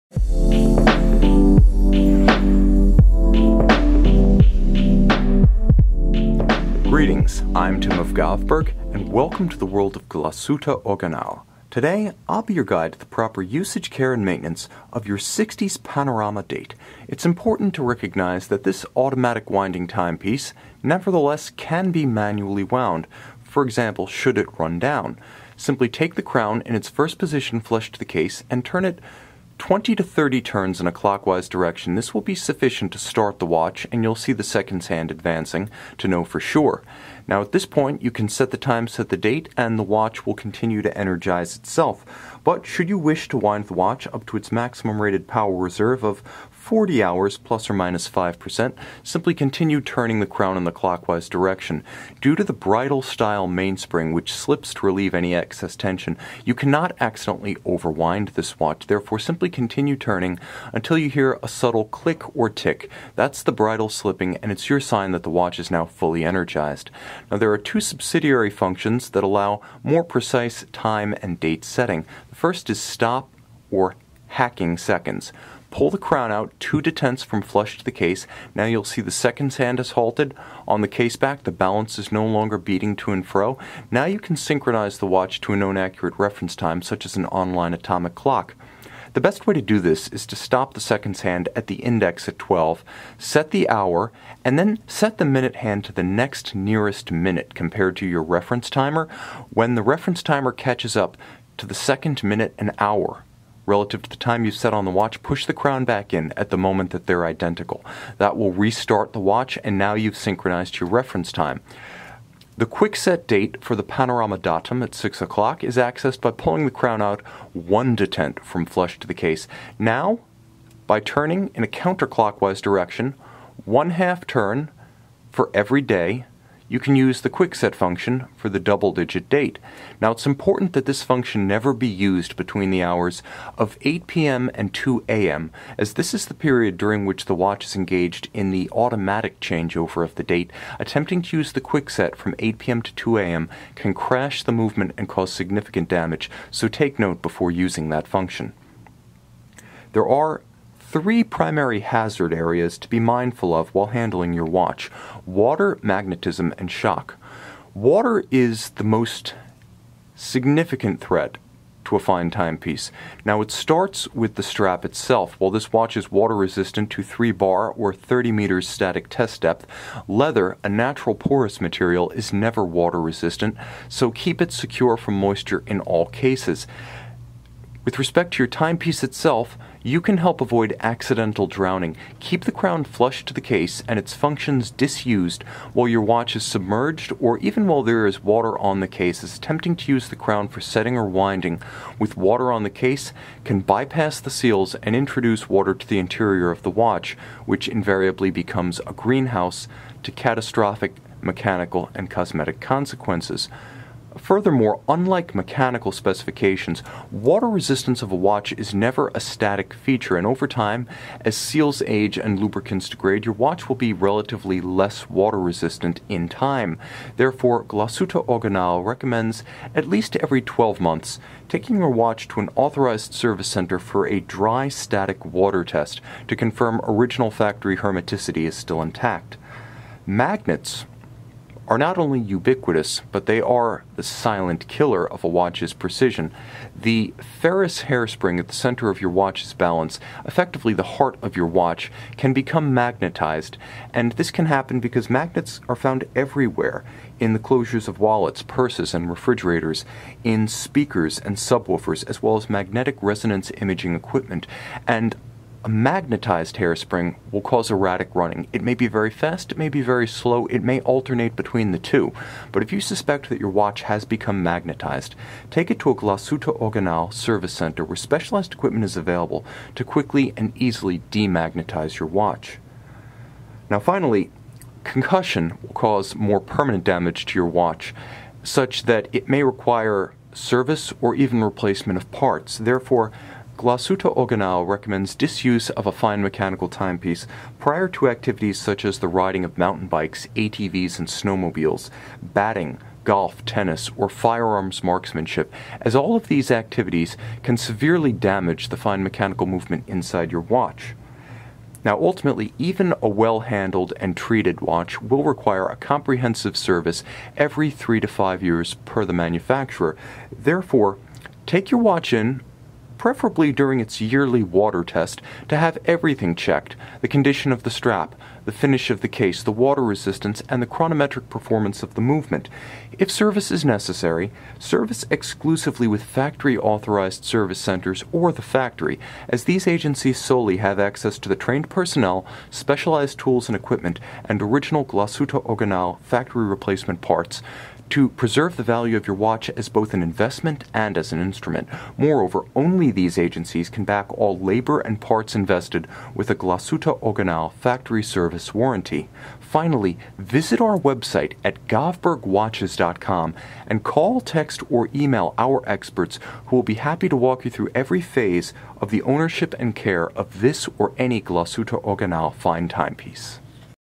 Glashutte Original Sixties Panorama Date 2-39-47-01-02-04 Functions and Care
Greetings, and welcome to the world of Glashütte Original. Today, I'll be your guide to the proper usage, care, and maintenance of your Sixties Panorama Date.

It's important to recognize that this automatic winding timepiece nevertheless can be manually wound, for example, should it run down. Simply take the crown in its first position, flush to the case, and turn it 20 to 30 turns in a clockwise direction. This will be sufficient to start the watch, and you'll see the seconds hand advancing to know for sure. Now, at this point you can set the time, set the date, and the watch will continue to energize itself. But should you wish to wind the watch up to its maximum rated power reserve of 40 hours, plus or minus 5%, simply continue turning the crown in the clockwise direction.

Now, there are two subsidiary functions that allow more precise time and date setting. The first is stop or hacking seconds. Pull the crown out two detents from flush to the case. Now you'll see the seconds hand has halted. On the case back, the balance is no longer beating to and fro. Now you can synchronize the watch to a known accurate reference time, such as an online atomic clock.

The quick set date for the Panorama Date, at 6:00, is accessed by pulling the crown out one detent from flush to the case. Now by turning in a counterclockwise direction, one half turn for every day, you can use the quick set function for the double digit date. Now it's important that this function never be used between the hours of 8PM and 2 AM, as this is the period during which the watch is engaged in the automatic changeover of the date. Attempting to use the quick set from 8PM to 2AM can crash the movement and cause significant damage, so take note before using that function.

There are three primary hazard areas to be mindful of while handling your watch. Water, magnetism, and shock. Water is the most significant threat to a fine timepiece. Now it starts with the strap itself. While this watch is water resistant to three bar, or 30 meters static test depth, leather, a natural porous material, is never water resistant. So keep it secure from moisture in all cases. With respect to your timepiece itself, you can help avoid accidental drowning. Keep the crown flush to the case and its functions disused while your watch is submerged or even while there is water on the case. It's tempting to use the crown for setting or winding with water on the case, can bypass the seals and introduce water to the interior of the watch, which invariably becomes a greenhouse to catastrophic mechanical and cosmetic consequences.

Magnets are not only ubiquitous, but they are the silent killer of a watch's precision. The [inaudible 00:03:57] hairspring at the center of your watch's balance, effectively the heart of your watch, can become magnetized. This can happen because magnets are found everywhere: in the closures of wallets, purses, and refrigerators, in speakers and sub woofers, as well as magnetic resonance imaging equipment. A magnetized hairspring will cause erratic running. It may be very fast, it may be very slow, it may alternate between the two. But if you suspect that your watch has become magnetized, take it to a Glashütte Original service center where specialized equipment is available to quickly and easily de-magnetize your watch.

Now finally, concussion can cause more permanent damage to your watch, such that it may require service or even replacement of parts. Therefore, Glashütte Original recommends disuse of a fine mechanical timepiece prior to activities such as the riding of mountain bikes, ATVs and snowmobiles, batting, golf, tennis, or firearms marksmanship, as all of these activities can severely damage the fine mechanical movement inside your watch.

Now ultimately, even a well-handled and treated watch will require a comprehensive service every three to five years per the manufacturer; therefore, take your watch in, preferably during its yearly water test, to have everything checked. The condition of the strap, the finish of the case, the water resistance, and the chronometric performance of the movement. If service is necessary, service exclusively with factory authorized service centers or the factory, as these agencies solely have access to the trained personnel, specialized tools and equipment, and original Glashütte Original factory replacement parts to preserve the value of your watch as both an investment and as an instrument. Moreover, only these agencies can back all labor and parts invested with a Glashütte Original factory service warranty.

Finally, visit our website at govbergwatches.com and call, text, or email our experts who will be happy to walk you through every phase of the ownership and care of this or any Glashütte Original fine timepiece.